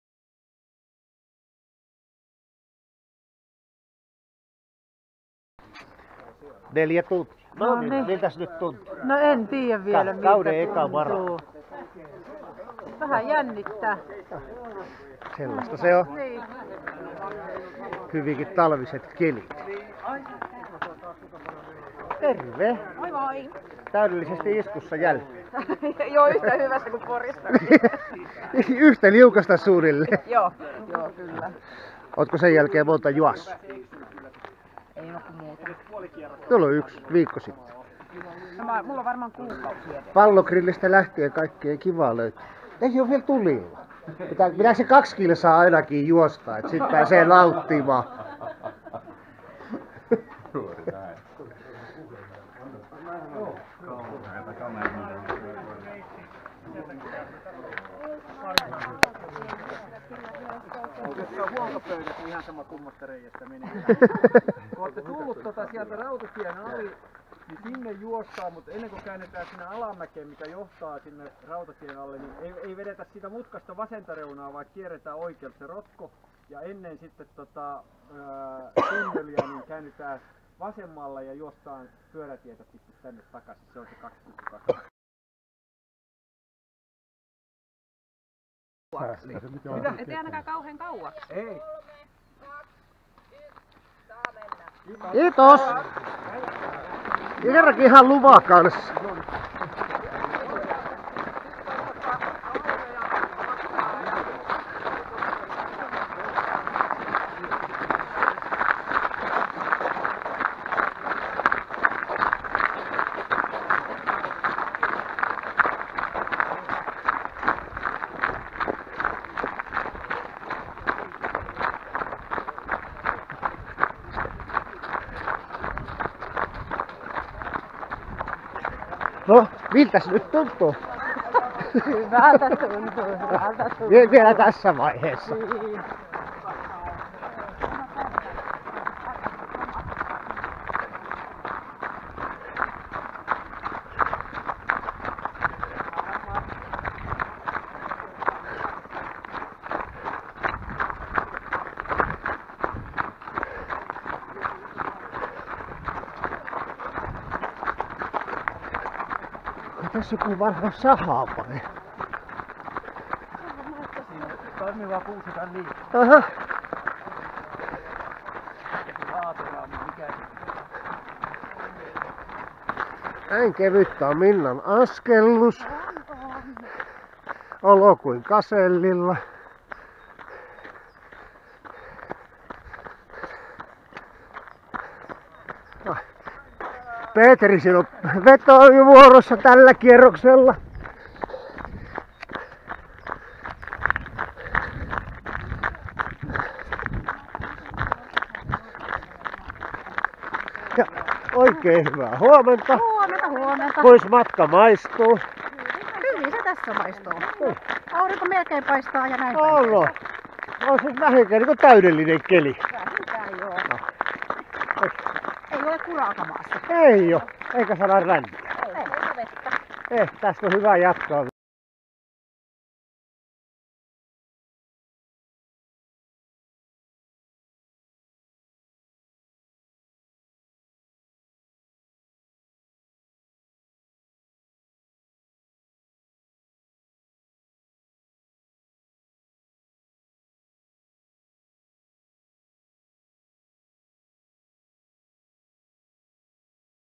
Petrin 300. juhlamaraton Kangasala 7.3.2020
Minnan kanssa kerrankin päästiin yhteislenkille ja häne ukkosa oli meille viel kuskinakin joten iha A luokan reissu! Nautiskelijamaratoonarit ry:n toimesta oma kymmenes mara tuli suoritettua ja samalla seitsemännessä kunnassa kotimaassa maran hölkkäsin.